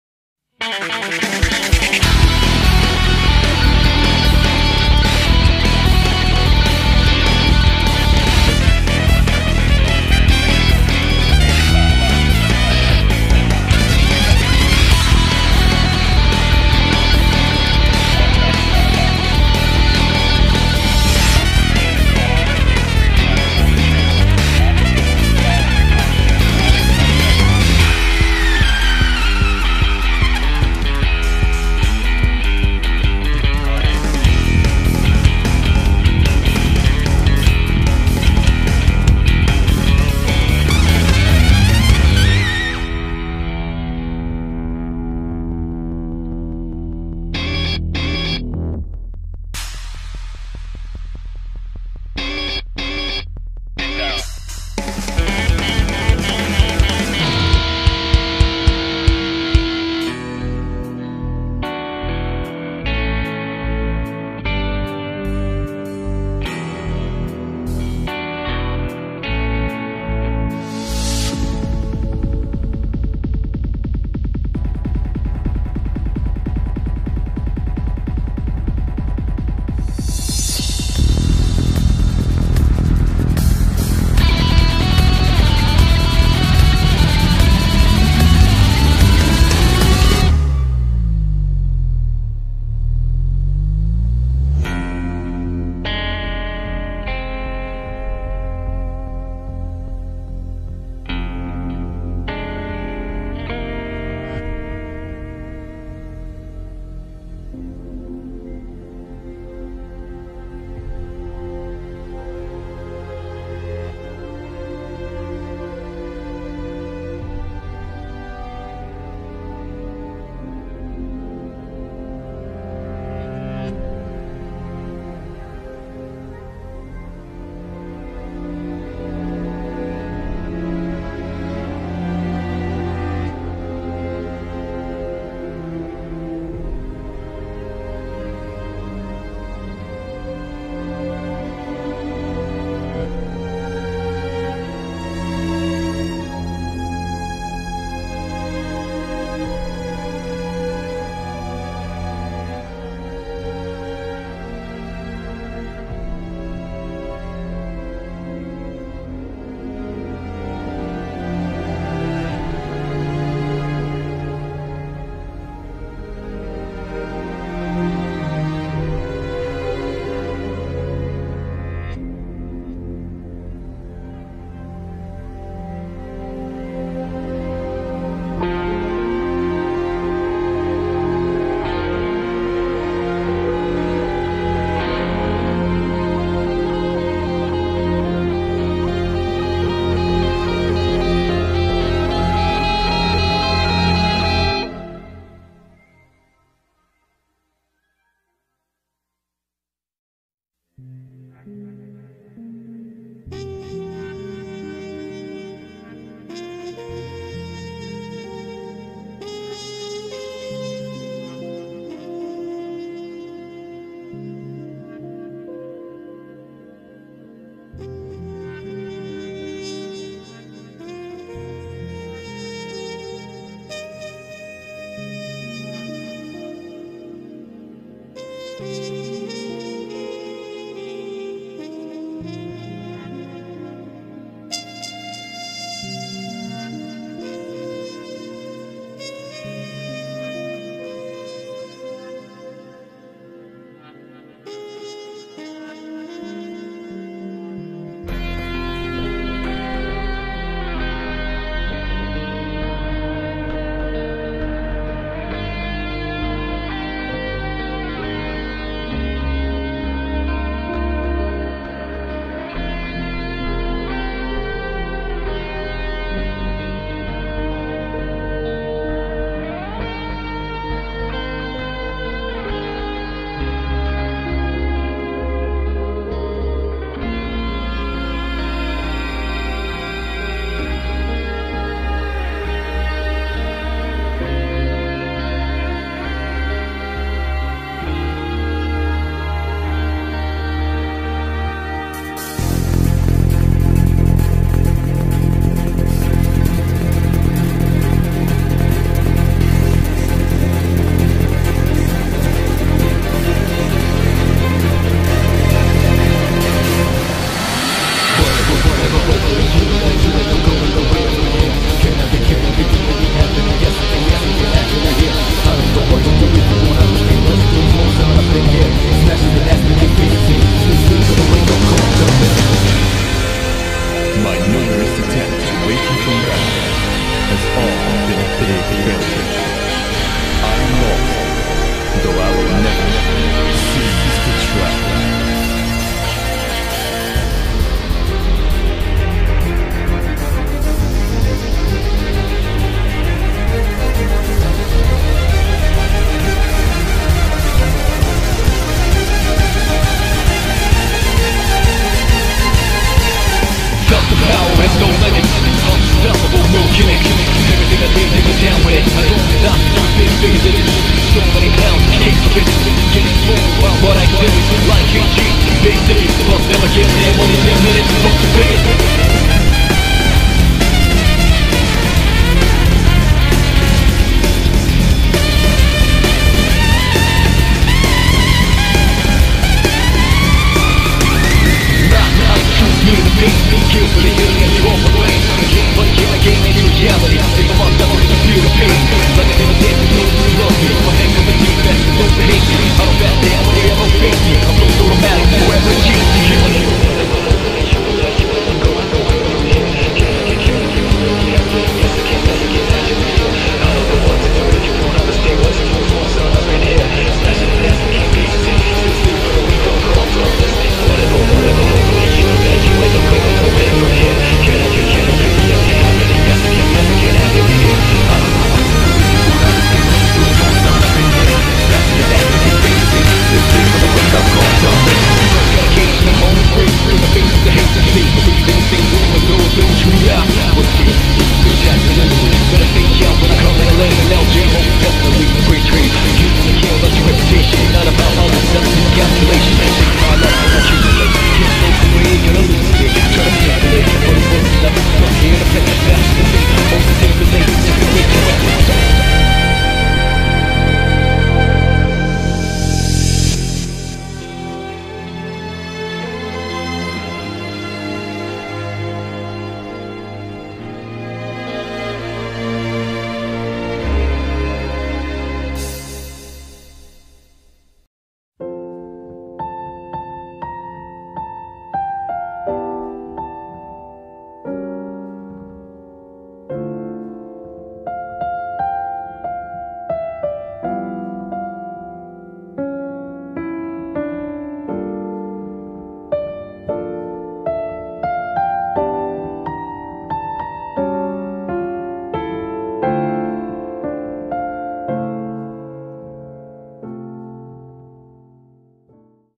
BSD Speedpaint - Fyodor Dostoyevsky
fyodor! :3c i love this smug rat man
consistent schedule who? publishing this super early bc im not gonna have access to a computer for a while

music listed at the end of the video!